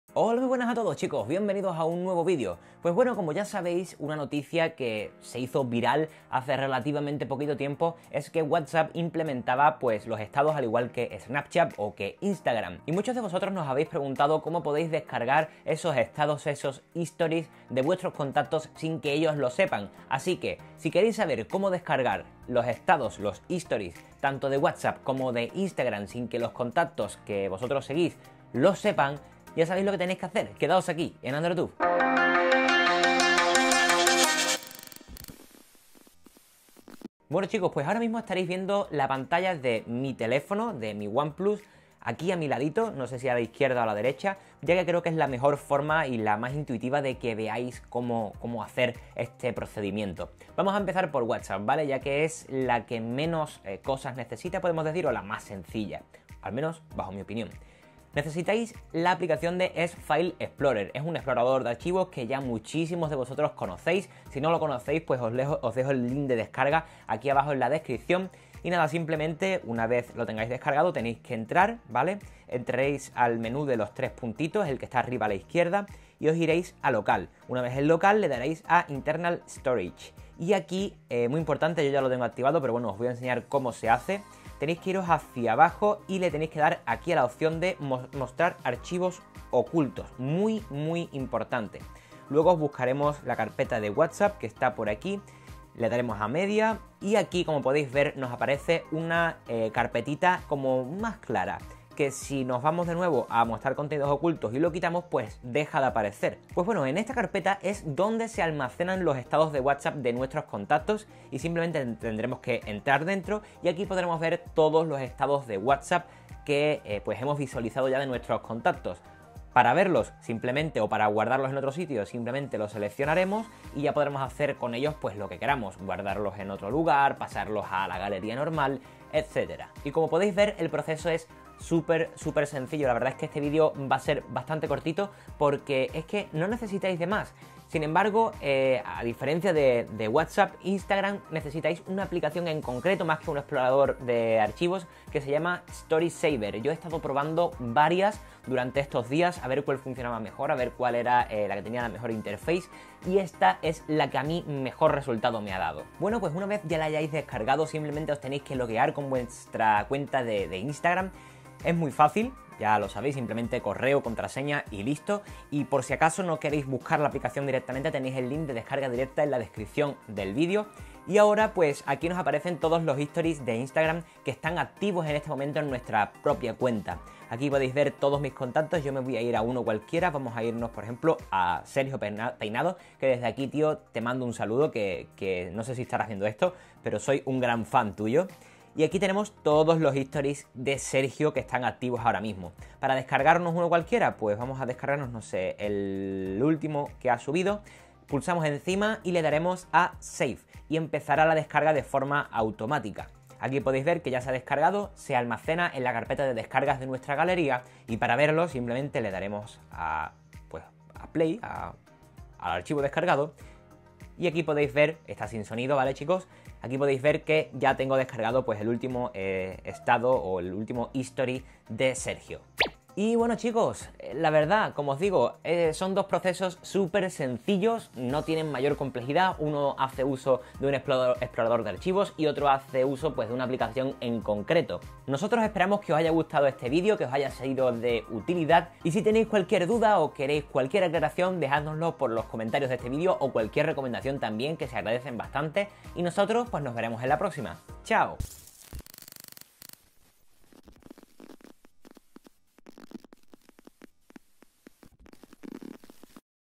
Descargar historias de Instagram y Whatsapp en ANDROID
En este vídeo os explicaremos a descargar historias de Instagram y Whatsapp en ANDROID de la forma más fácil y rápida.

Si te ha gustado "" no olvides apoyarnos con tu Like!

●VÍDEOS QUE TE INTERESAN●